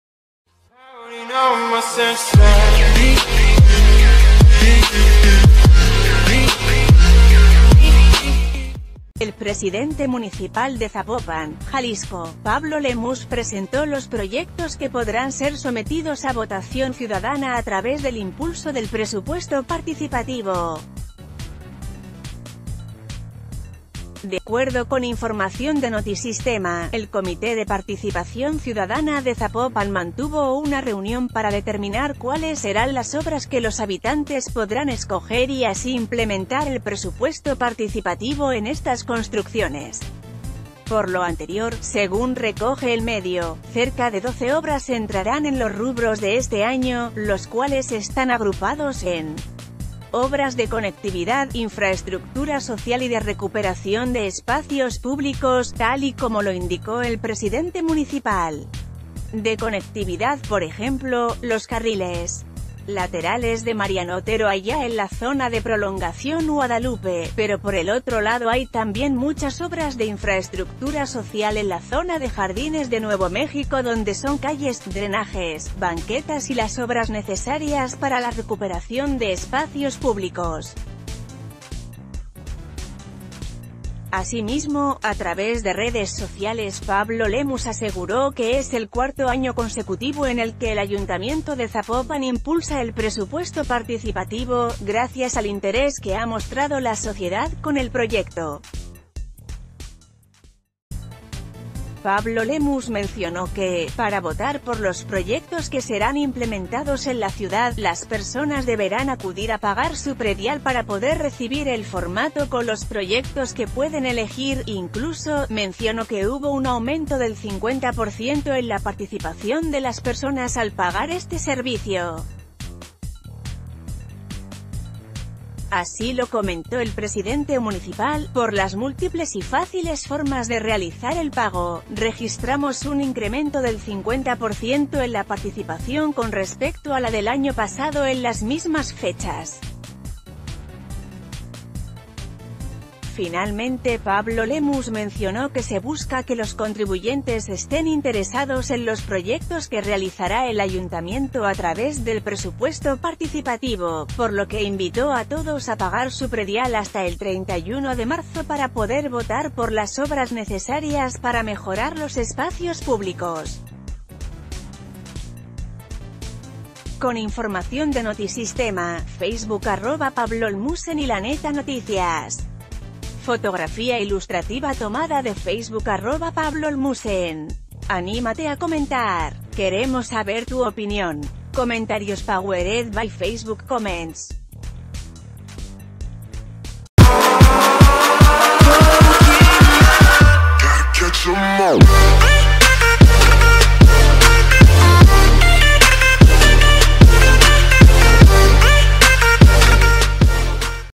Presupuesto Participativo: Lemus presenta 12 obras a elección de ciudadanos
El presidente municipal de Zapopan, Jalisco, Pablo Lemus, presentó los proyectos que podrán ser sometidos a votación ciudadana a través del impulso del Presupuesto Participativo. De acuerdo con información de Notisistema, el Comité de Participación Ciudadana de Zapopan mantuvo una reunión para deter...